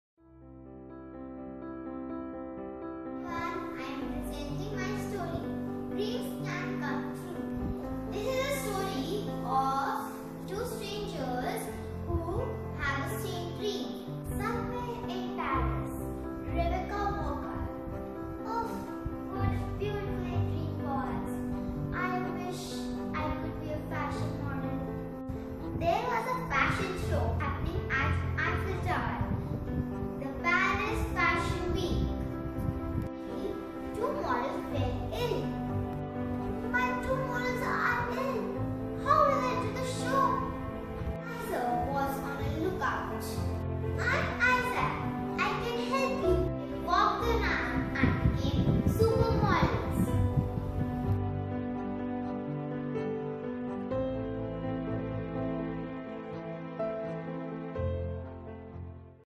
Story Creation I Presentation I Mind Map I Speech and Drama
CREATE . NARRATE . SOLO ACT  Story Creation and Presentation. Age Group:  8-11 Years. A pictorial inspiration helped 'Create' a story. Structured thinking through Mind Map led to a Presentation', which further challenged the particpant to 'Narrate' and enact a 'Solo Act'. Kudos! 😀👏